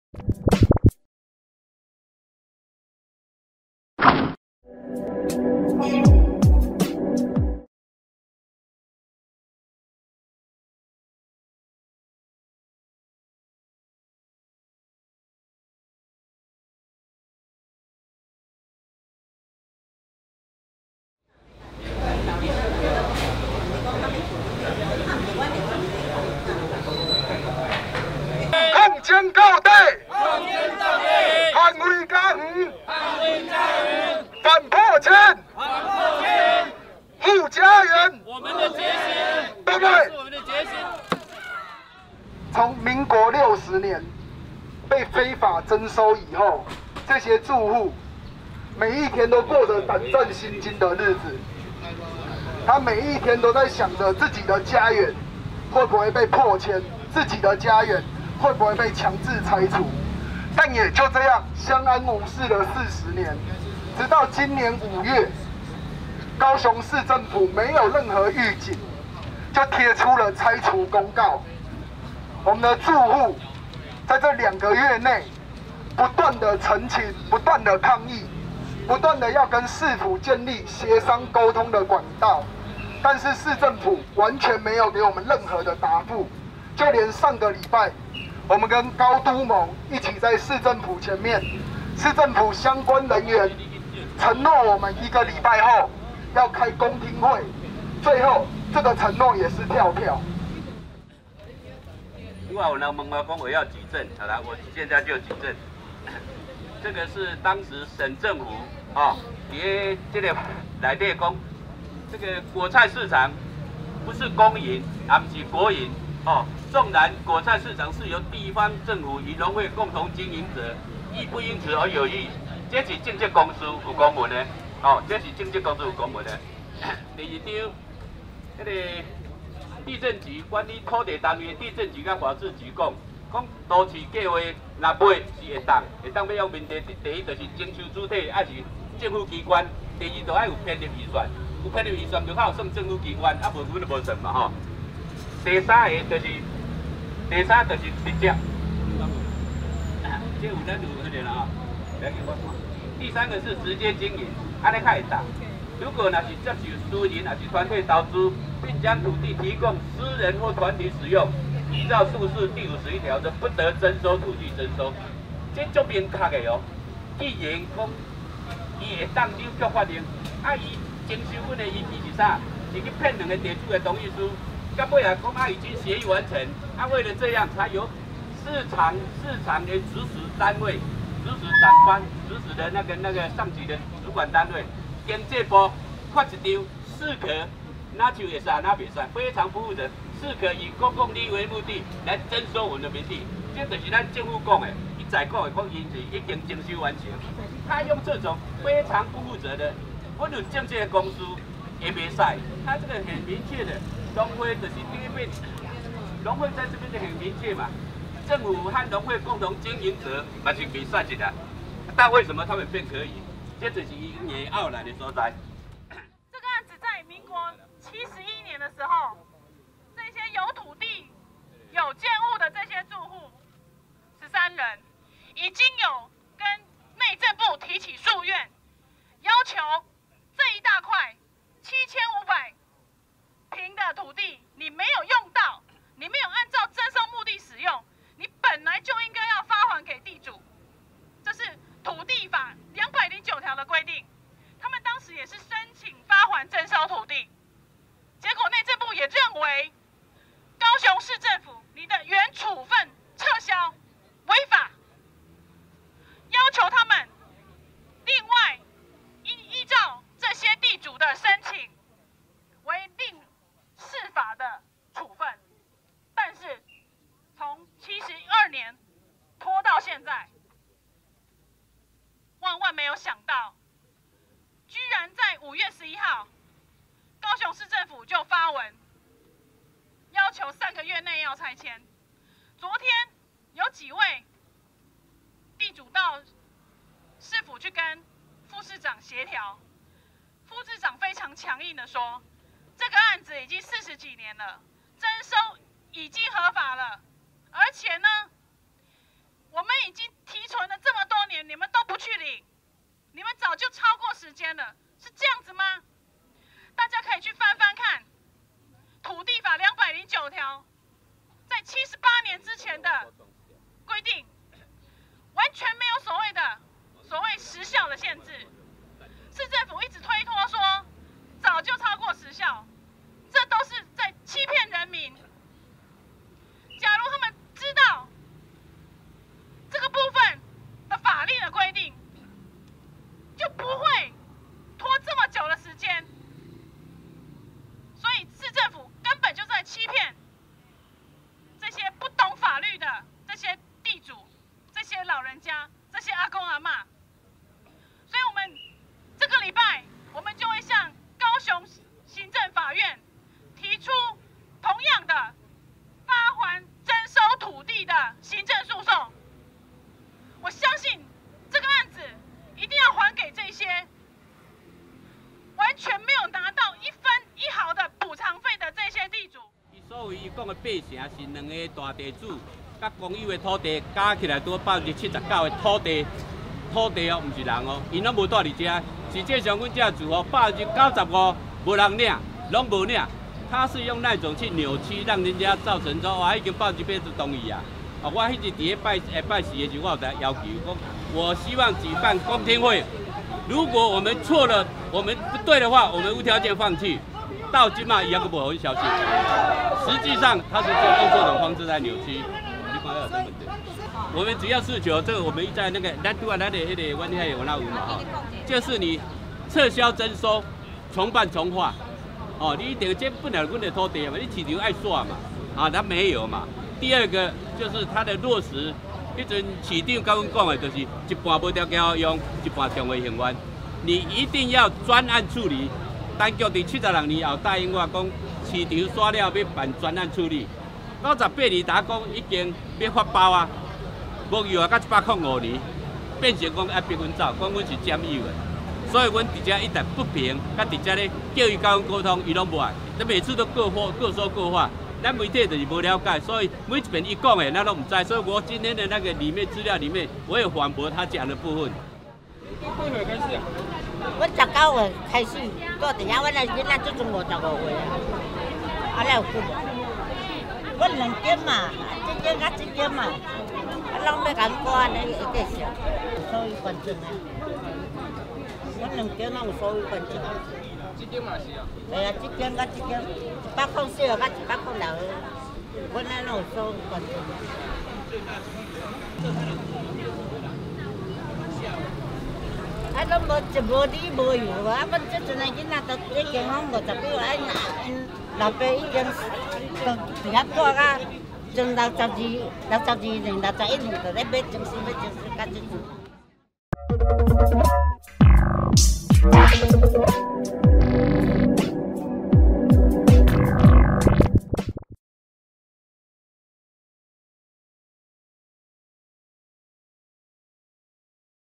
2016.07.27高雄果菜市場自救會斬雞頭抗議 駁市府對徵收案說法
昨(27日)上午，高雄市果菜市場不義徵收住戶自救會於高雄民族果菜市場旁舉辦記者會，以斬雞頭、燒黃紙宣示抗爭到底的決心，也出面駁斥高市府近日對於果菜市場徵收案的對外說法。自救會代表吳富雄指出市府對外宣稱住戶已願意自動拆遷、八成地主已經同意等等是混淆視聽的說法，事實上所謂八成地主是指兩名地主加上公有地的數目，從未有住戶同意徵收。義務律師柯邵臻也宣告，將於下周再度提起撤銷土地徵收的行政訴訟，希望法院能還居民一個公道。自救會助理曾維堂也說明，近兩個月自救會曾多次陳情，市府也同意將召開公聽會，但也跳票未舉辦。